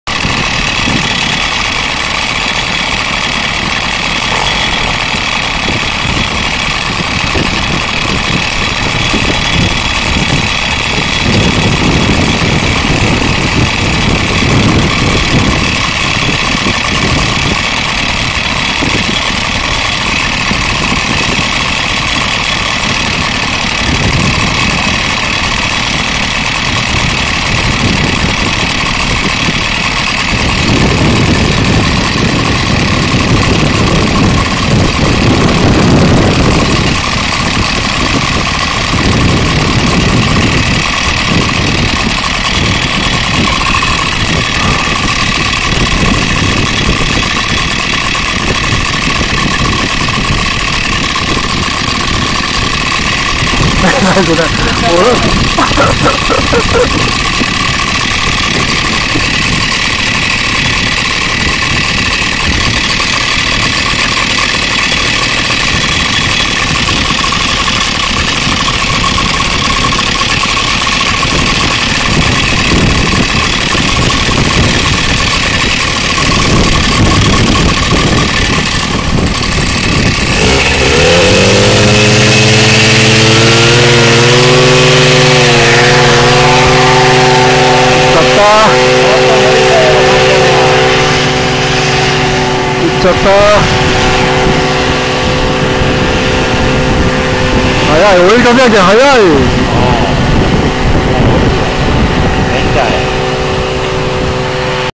paramotor take-off 土佐清水 大岐の浜２ 赤鉄橋 四万十川
20100606　土佐清水市　大岐海岸から　take-off　しました、　海風は　強くても　安定性　抜群　揺れませんでした、　海風　最高です、　パイロットはＫ葉　さん　さすが　です。